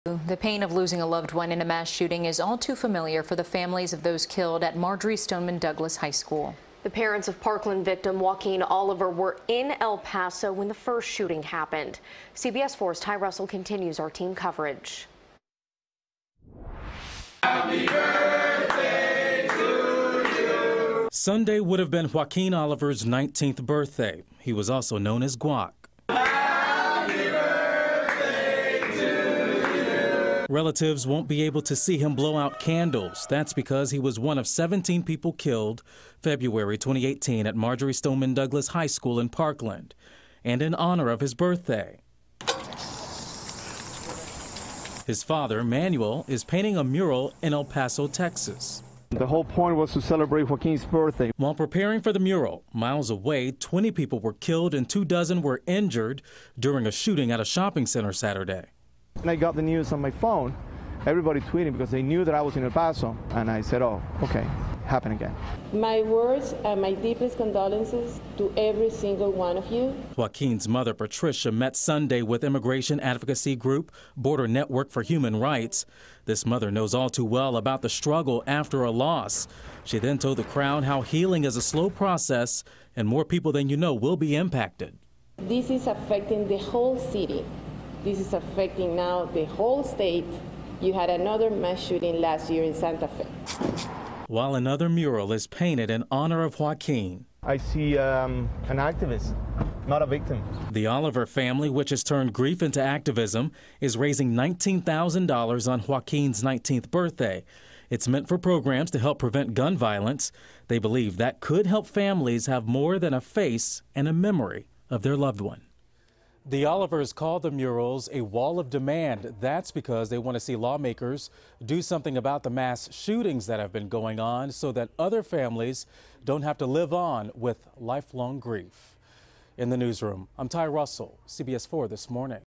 Parents Of Parkland Shooting Victim In El Paso
Ty Russell reports the parents of Parkland victim Joaquin Oliver were in El Paso when a man killed at least 20 people and injured dozens more near a shopping mall on Saturday.